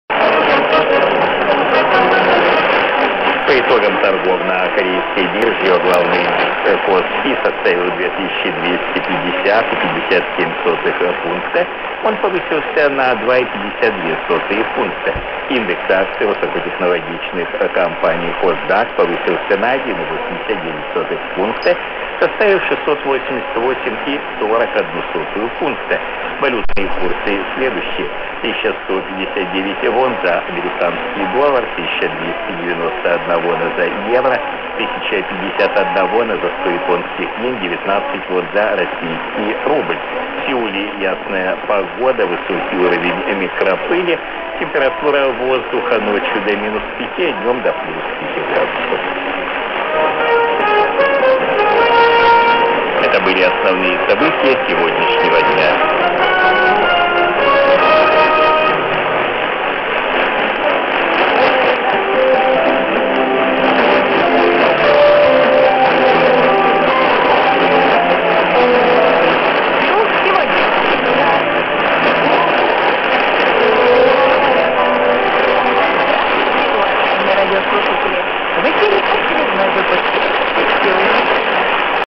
KBS World Radio via HLSR 1170 kHz
Russian program
TX: 500 kW Gimje, South Korea
QTH: Northwestern Siberia of Russia (MP87DA)
2020/01/17